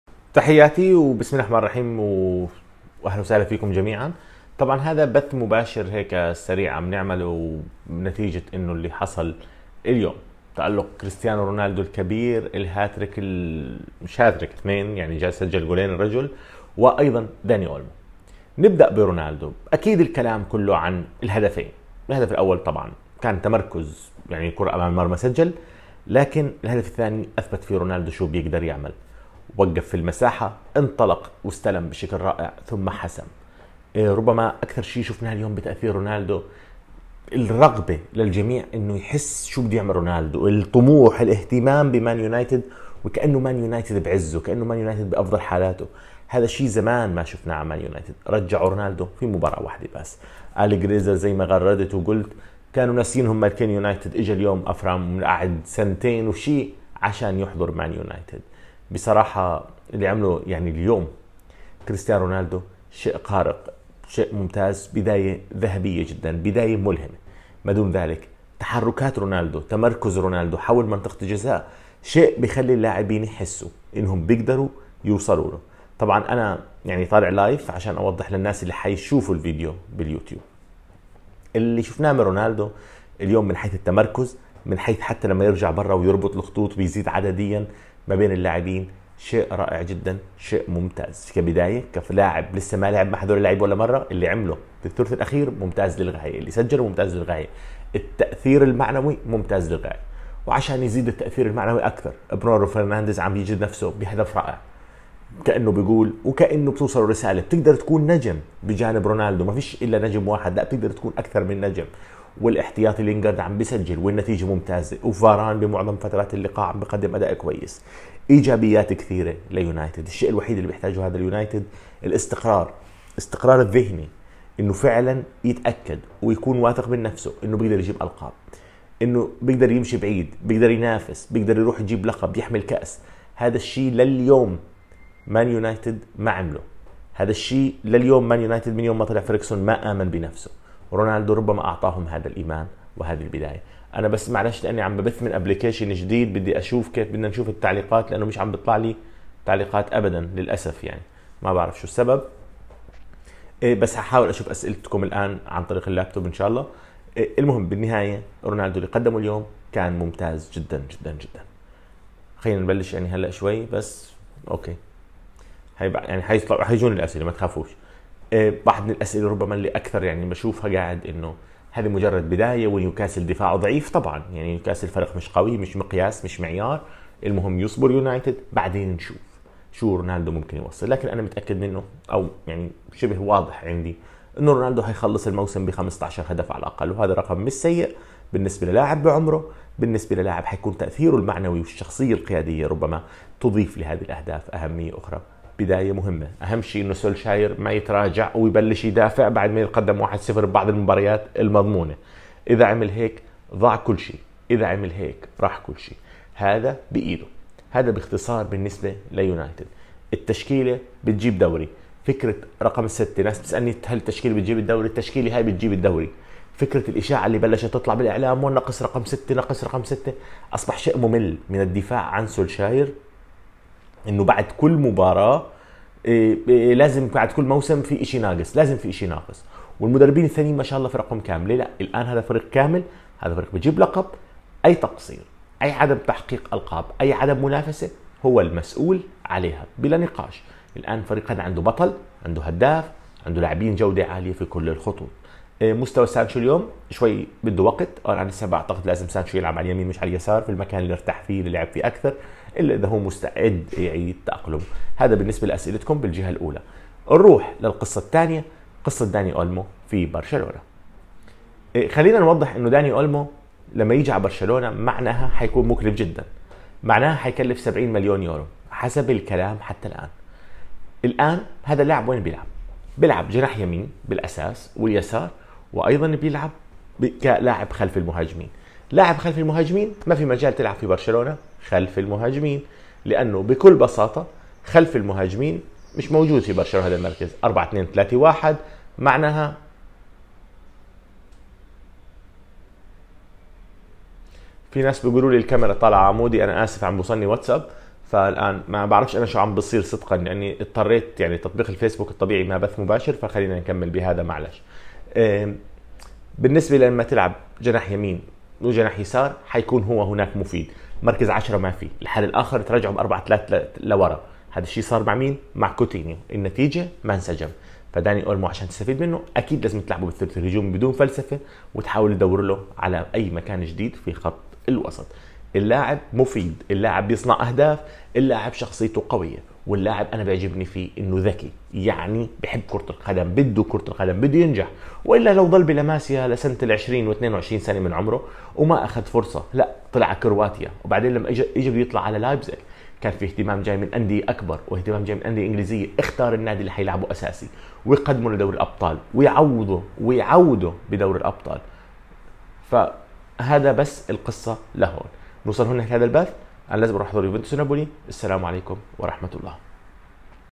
كريستيانو رونالدو يتألق ضد نيوكاسل - أولمو وبرشلونة
فيديو بث مباشر بعد تألق كريستيانو رونالدو .. اقتراب داني أولمو من برشلونة .

حساباتي في شبكات التواصل:
تيلغرام :